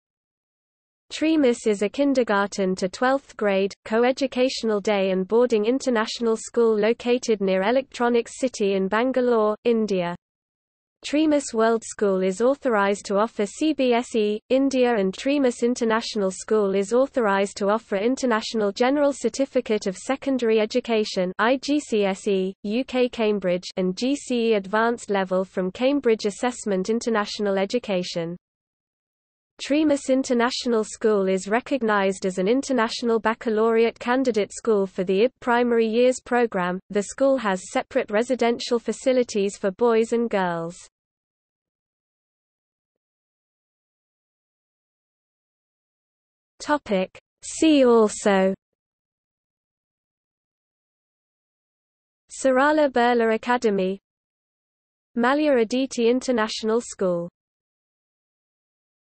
Treamis World School | Wikipedia audio article
00:00:42 See also

- Socrates

SUMMARY
Treamis is a Kindergarten to 12th grade, co-educational day and boarding international school located near Electronics City in Bangalore, India.  Treamis World School is authorized to offer CBSE, India and Treamis International School is authorized to offer International General Certificate of Secondary Education (IGCSE, UK-Cambridge) and GCE Advanced Level from Cambridge Assessment International Education.
Treamis International School is recognized as an International Baccalaureate candidate school for the IB Primary Years Programme.The school has separate residential facilities for boys and girls.